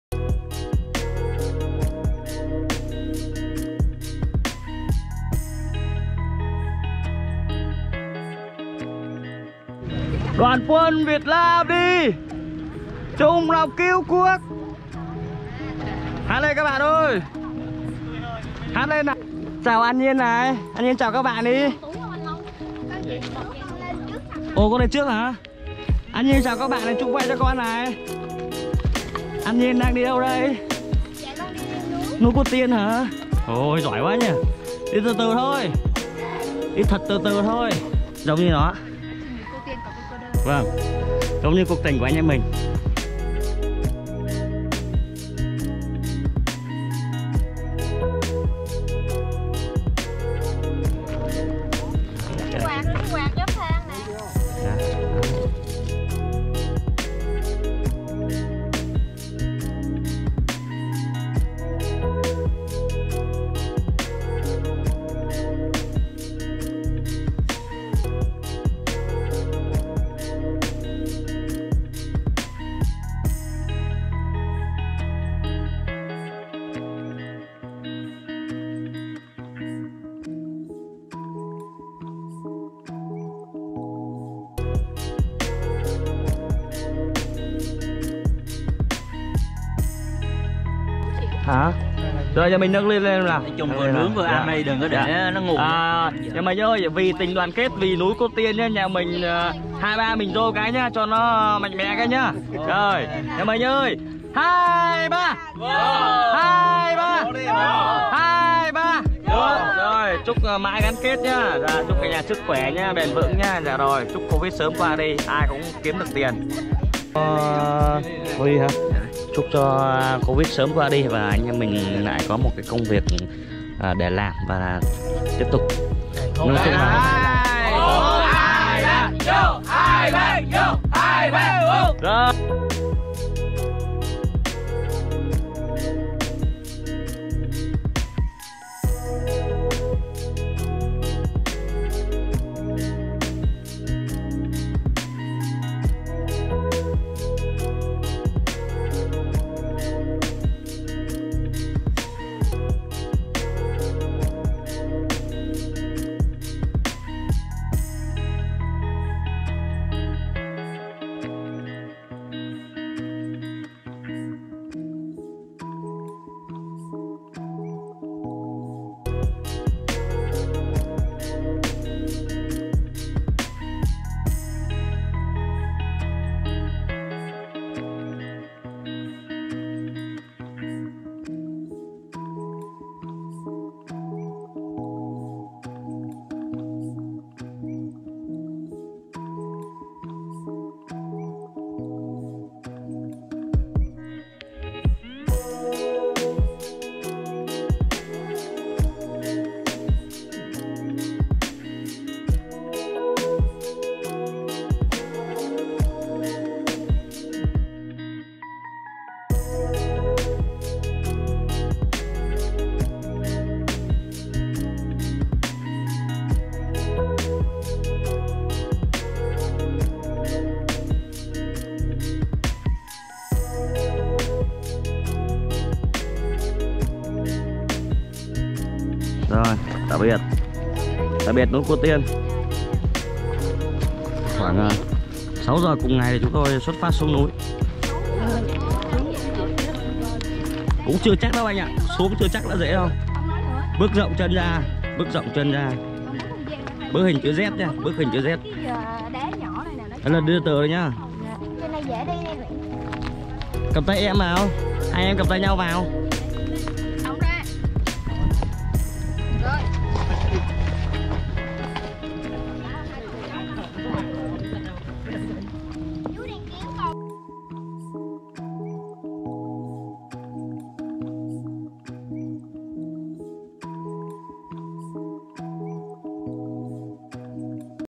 Núi Cô Tiên - Kỷ Niệm 29/05/2021 | Nha Trang Trong Tôi
Leo núi Cô Tiên để “chinh phục” thiên nhiên Nha Trang tuyệt đẹp

Du lịch Nha Trang không chỉ có mỗi biển xanh, cát trắng, nắng vàng mà du khách còn có thể khám phá những ngọn núi tuyệt đẹp và núi Cô Tiên là một trong những hòn núi như thế. Bạn sẽ cảm thấy vô cùng thích thú khi đứng trên đỉnh cao nhất và nhìn ra toàn cảnh thành phố biển đẹp hút hồn này đấy.